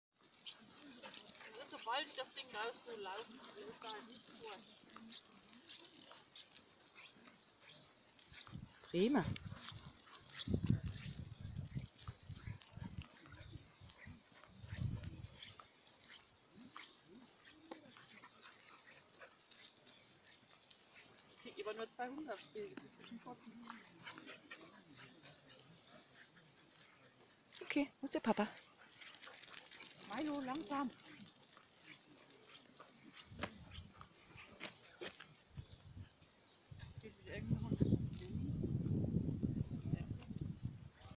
Mai 2019 - Spaziergang im Westerwald
Mailo mit seinem Bruder Buddy und Mama Sammy total entspannt beim Spaziergang.